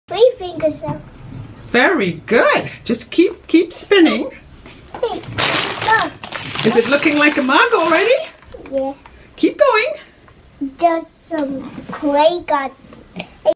Finnegan
Getting an idea as to how a potters Wheel would work until the real thing arrives